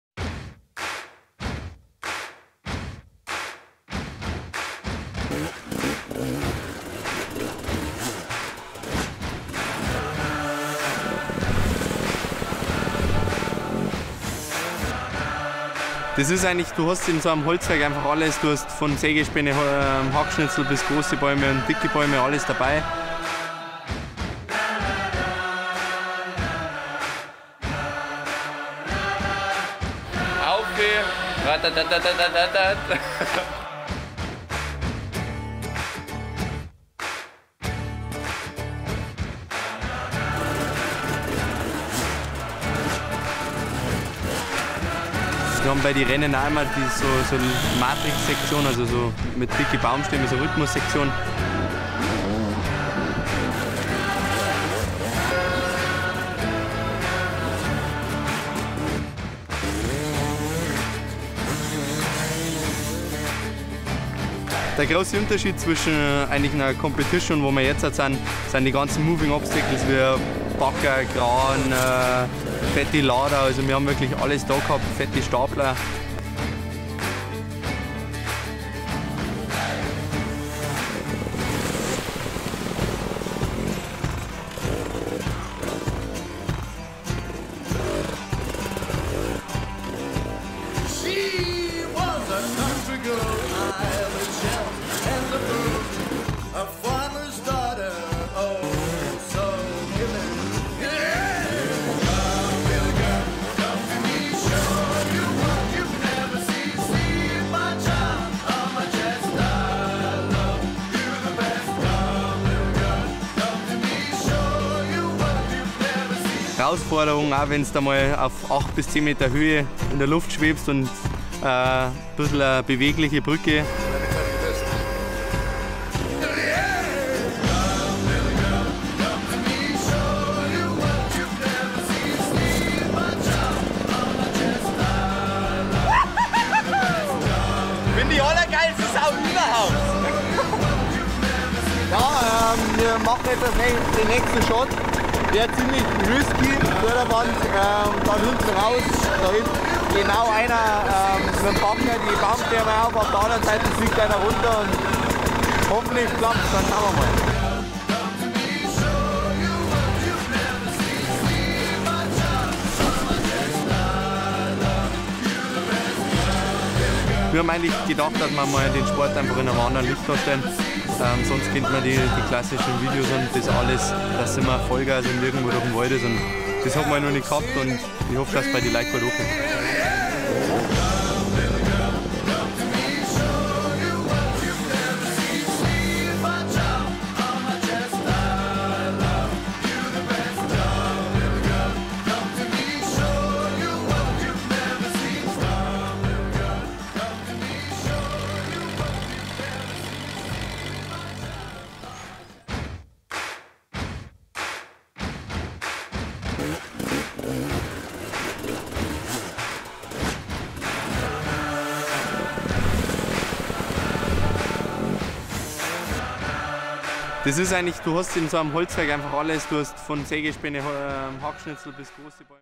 Behind the scenes of Sawmill Enduro w/ Manuel Lettenbichler
Behind the Scenes of the XXL off-road experience: One of Europe's largest sawmills as an oversized log section playground gives the Enduro rider Manuel Lettenbichler a training of a special kind.

Drifting curves on mountains of sawdust, balancing on towering, never-ending tree-stacks, adventurous encounters with gigantic machines - Super Enduro junior champion Manuel Lettenbichler looks for spectacular tracks in the Binderholz sawmill in Fügen in Zillertal (Tyrol, Austria) and showcases his extraordinary talent.

No question, the 20-year-old youngster is carved out of the right wood, since it comes from the line of the legendary Enduro star Andi Lettenbichler. With his background as a trail biker, the kid out of Kiefersfelden (Germany) proves his riding skills on a terrain that is unparalleled to anything seen. The surface changes constantly, steep climbs on slippery bark alternate with dusty sawdust, he races through narrow passages from piles of boards, uses tree trunks while they are loaded by huge machines as bridges and drives at high speed next gigantic saws along the production line.

It's just these huge machines that build up tons of obstacles in minutes, set ramps and landings, or allow them to balance on a floating log for the Enduro Pro under the motto "Let's work together". Manuel Lettenbichler leaves nothing untouched in this sawmill of superlatives: on a site of about 40 hectares that contains about 70,000 tonnes wood, which is supplied with more than 150 truckloads a day.